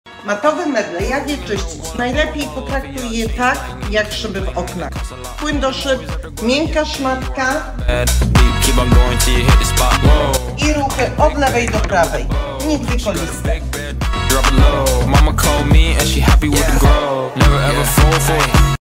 🔘 Matowe meble - jak je czyścić? 🤔
🤔  Matowe meble - jak je czyścić? Zastosuj te kilka porad, a będziesz cieszyć się pięknymi, matowymi powierzchniami! 👌

🔹 stosuj płyn do szyb
🔹 użyj miękkiej szmatki
🔹 pamiętaj, ruchy od lewej do prawej!

❌ NIGDY koliste! Czy wiesz dlaczego? 🤔